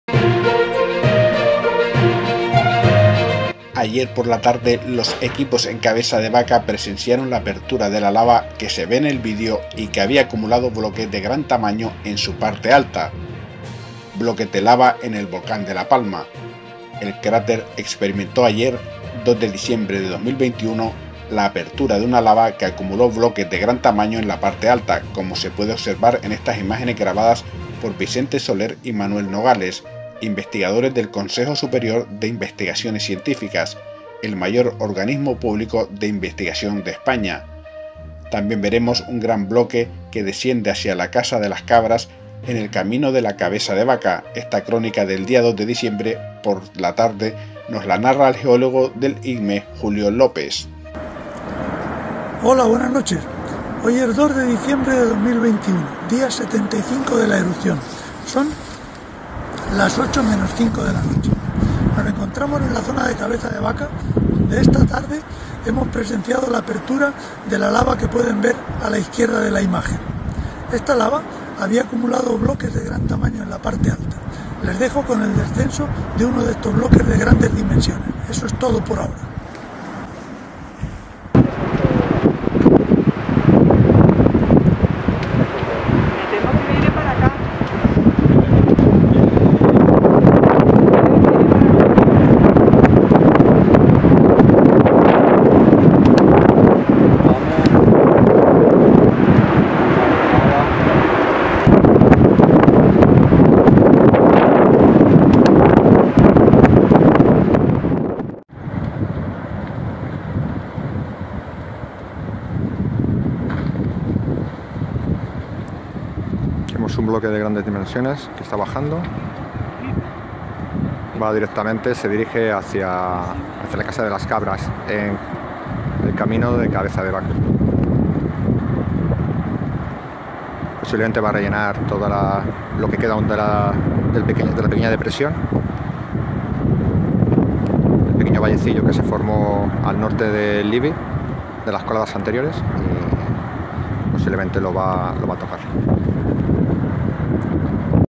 🟥🌋BLOQUES GIGANTES DE LAVA🌋🟥 expulsados por el volcán de La Palma
🟡🟡🟡CORRECIONES del vídeo: 0:00 - 0:28 s | Las imágenes que se ven en este fragmento fueron grabadas por Vicente Soler (IPNA-CSIC) y Antonio González (Cabildo de La Palma), no por "Vicente Soler y Manuel Nogales", que es lo que se menciona en el vídeo.
0:29 - 0:49 |Se visualiza una secuencia grabada por el IGME-CSIC que aparece justo después de la mención anterior, lo que puede generar un error de atribución.
0:50 s | La voz en off dice "nos la narra el geólogo del IGME Julio López" y comienza una crónica de Vicente Soler (IPNA-CSIC) como bien puede leerse en la rotulación en la esquina inferior izquierda.
1:48 s | Comienza un vídeo grabado por personal del IGME-CSIC y se oye una descripción de una voz en off que sí se corresponde con la Julio López (IGME-CSIC) pero aparece justo después de la crónica anterior, sin ningún tipo de rotulación que las diferencie.

🟡🟡🟡Ayer por la tarde los equipos en Cabeza de Vaca presenciaron la apertura de la lava que se ve en el vídeo y que había acumulado bloques de gran tamaño en su parte alta.

🟡Bloques de lava en el volcán de La Palma.

🟡El cráter experimentó ayer (02/12/2021) la apertura de una lava que acumuló bloques de gran tamaño en la parte alta, como se puede observar en estas imágenes grabadas por Vicente Soler y Manuel Nogales, investigadores del Consejo Superior de Investigaciones Científicas (CSIC), el mayor organismo público de investigación de España

🟡También veremos un gran bloque que desciende hacia la casa de las Cabras, en el camino de Cabeza de Vaca. Esta crónica del día 2 de diciembre por la tarde nos la narra el geólogo del IGME, Julio López.